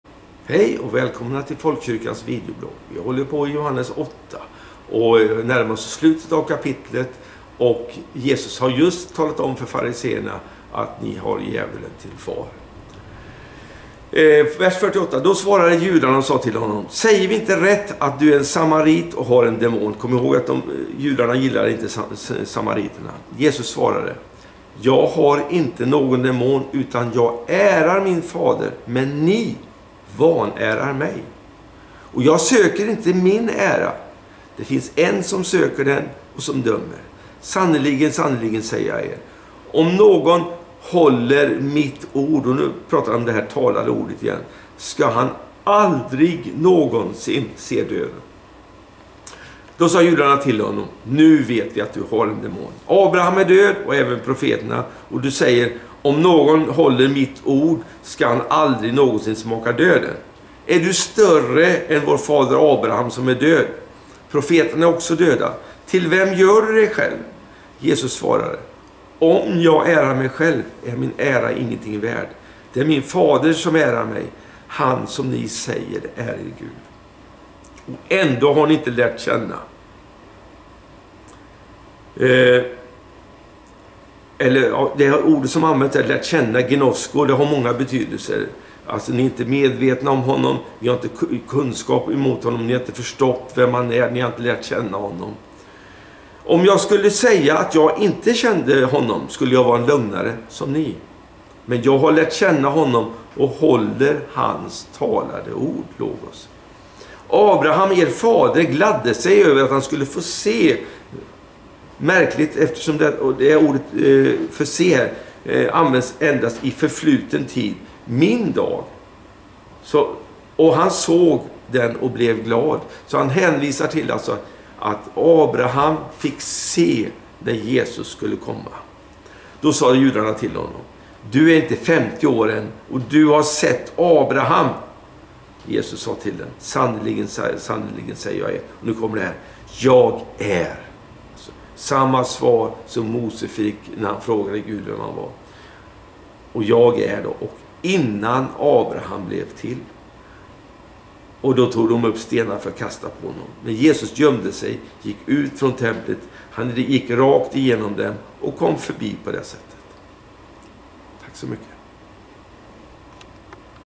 Videoblogg 26 februari 2021 med pastor Bertil Dolfén
I Folkkyrkans videoblogg 26 februari fortsätter pastor Bertil Dolfén tala om Johannesevangeliet. I avsnitt 44 har vi kommit fram till 8:48 "Judarna sade: 'Har vi inte rätt när vi säger att du är samarier och att du är besatt.' Jesus svarade: 'Jag är inte besatt utan jag ärar min fader, men ni skymfar mig. Jag söker inte min egen ära, men det finns en som söker den och som dömer. Sannerligen, jag säger er; den som bevarar mitt ord skall aldrig någonsin få se döden.' Judarna sade: 'Nu vet vi att du är besatt. Abraham dog och profterna likaså. Men du säger att den som bevarar ditt ord aldrig någonsin skall möta döden. Skulle du vara större än vår fader Abraham? Han dog och profeterna dog. Vem tror du att du är?' Jesus svarade: 'Om jag själv förhärligar mig, så är min härlighet ingenting värd. Det är min fader som förhärligar mig; han som ni säger är er Gud. Ni vet inte vem han är, men jag känner honom; och om jag säger att jag inte känner honom, så blir jag en lögnare som ni. Men jag känner honom och jag bevarar hans ord. Er fader Abraham jublade över att han skulle få se min dag. Han fick se den och gladde sig.' Judarna sade då till Jesus: 'Du är inte femtio år och ändå har du sett Abraham.' Han svarade: 'Sannerligen, jag säger er; jag är och jag var innan Abraham blev till.' Då tog de upp stenar för att kasta på honom, men Jesus försvann och lämnade templet."